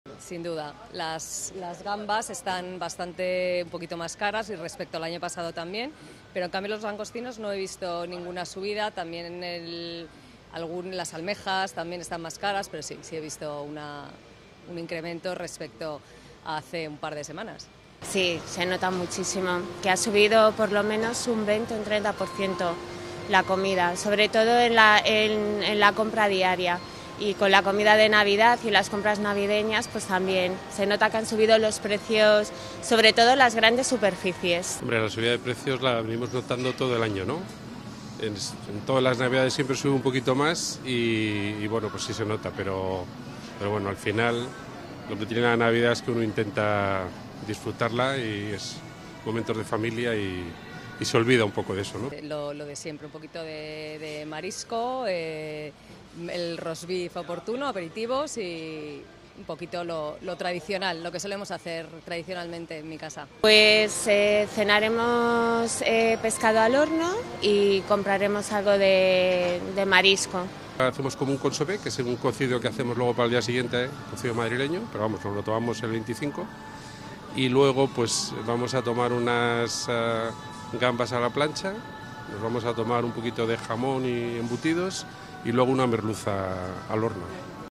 Clientes notan la subida de precios y comentan qué van a cenar en Nochebuena
Clientes en el Mercado de la Cebada (Madrid) han comentado que en los últimos días han notado una subida de precios generalizada en todas las superficies con la llegada de la Navidad y, por otra parte, han señalado qué van a cenar en Nochebuena.